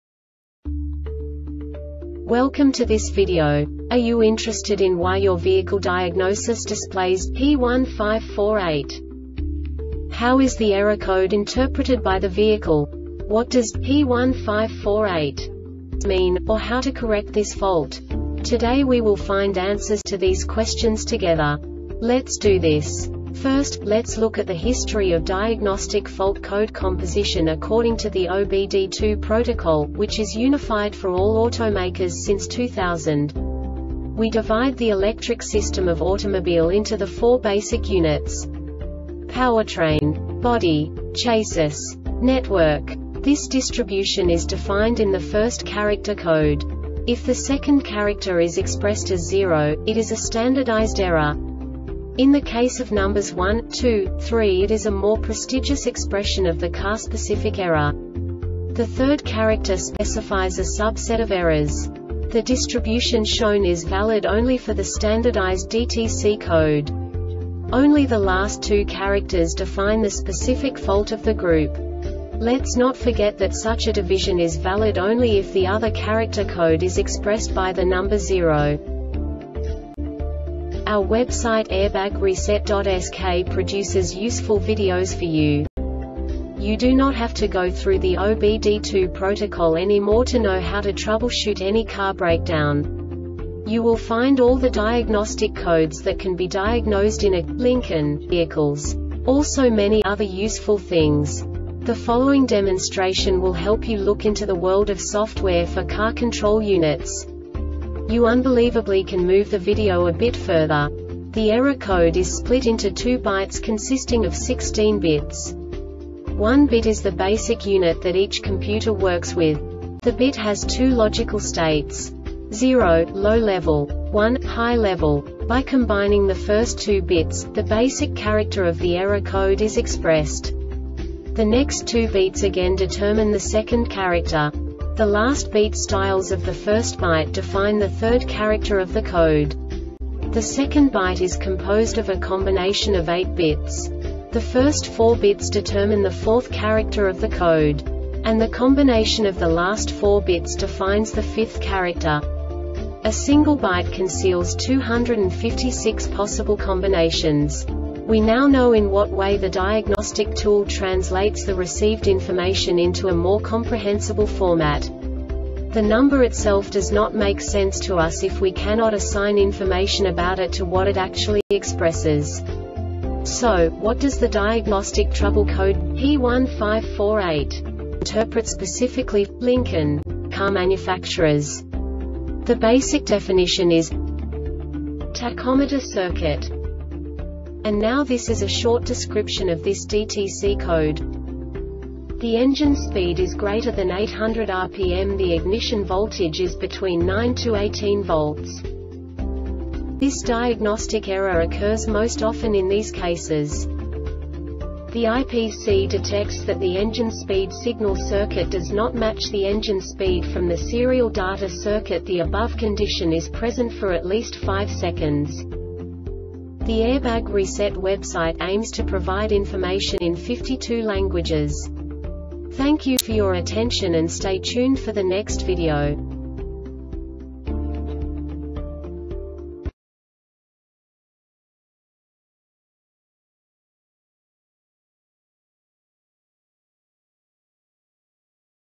DTC Lincoln P1548 Short Explanation
Contents:
0:21 Basic DTC analysis according to OBD2 protocol standard.
1:48 Insight into programming
2:58 A diagnostically understandable description of the Diagnostic Trouble Code
3:05 Basic error definition

Make
Lincoln
Code
P1548
Definition
Tachometer Circuit
Description
The engine speed is greater than 800 RPM  The ignition voltage is between 9-18 volts
Cause
The IPC detects that the engine speed signal circuit does not match the engine speed from the serial data circuitThe above condition is present for at least 5 seconds